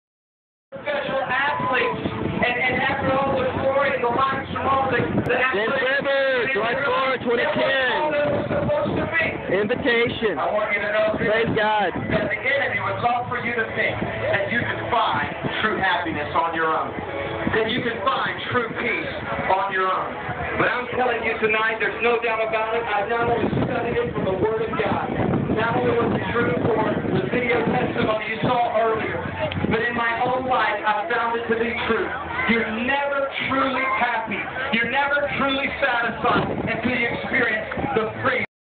July 4, 2010 8:29 PM
Public Invitation at the James River - I Love America fireworks celebration.